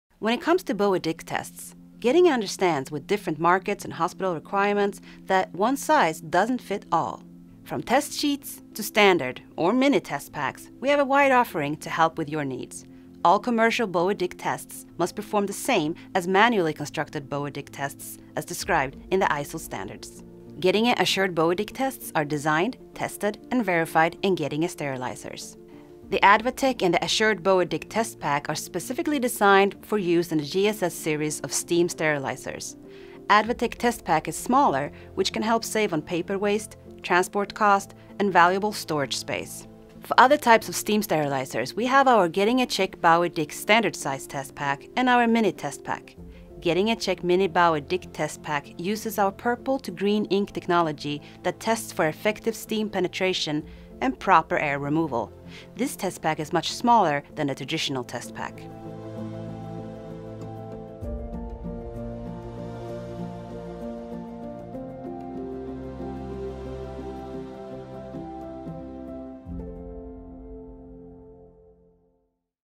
Getinge Assured Bowie-Dick tests
The Getinge Bowie-Dick Type Test is to be used for daily testing of the mechanical function of your vacuum assisted steam sterilizer, ensuring that its air removal system is working as expected.